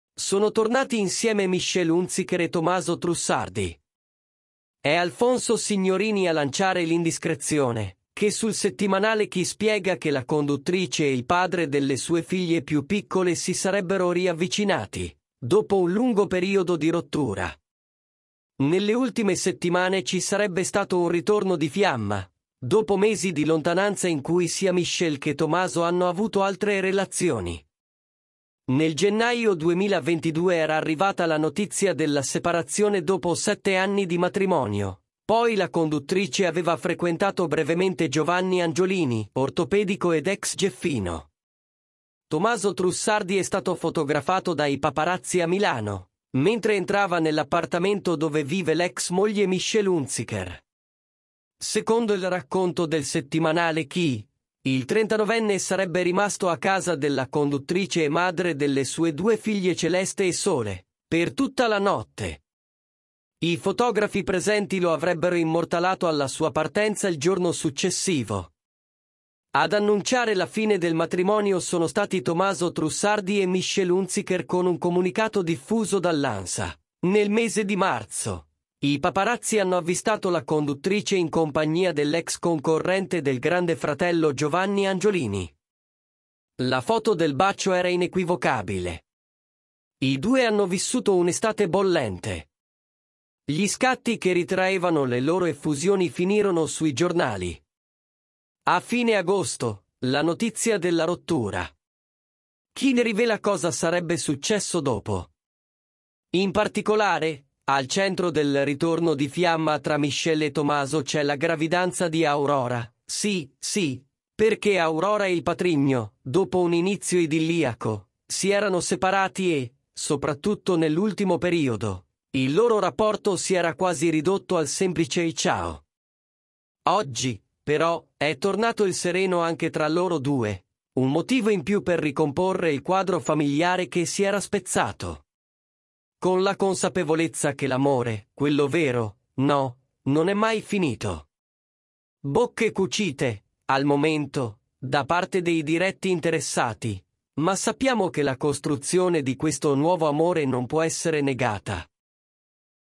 Michelle Hunziker E Tomaso Trussardi Sono Tornati Insieme? 'Ritorno Di Fiamma'
Sono tornati insieme Michelle Hunziker e Tomaso Trussardi. È Alfonso Signorini a lanciare l'indiscrezione, che sul settimanale Chi spiega che la conduttrice e il padre delle sue figlie più piccole si sarebbero riavvicinati dopo un lungo periodo di rottura. Nelle ultime settimane ci sarebbe stato un ritorno di fiamma, dopo mesi di lontananza in cui sia Michelle che Tomaso hanno avuto altre relazioni.
Nel gennaio 2022 era arrivata la notizia della separazione dopo sette anni di matrimonio, poi la conduttrice aveva frequentato brevemente Giovanni Angiolini, ortopedico ed ex gieffino.

❗IMPORTANTE❗
PER RICHIESTE DI COLLABORAZIONI PUBBLICITARIE:
invia una mail a e verrai contattato il prima possibile: